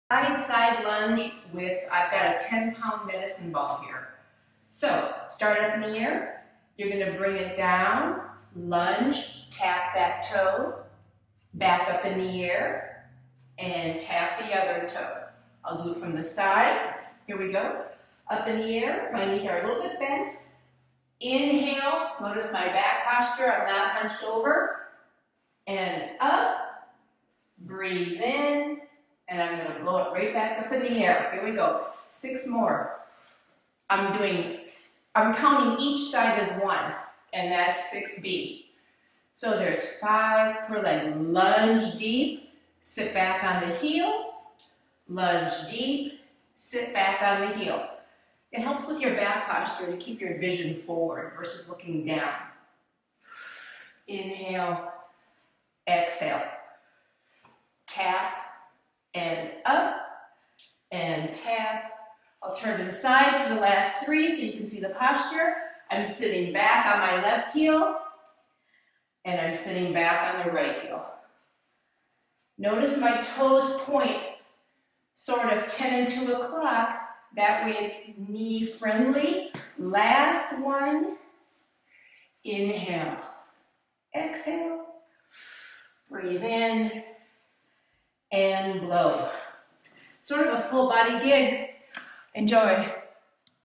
Medicine Ball For Intense Workout
Using a medicine ball for intensity, tone your arms, legs and core in this segment.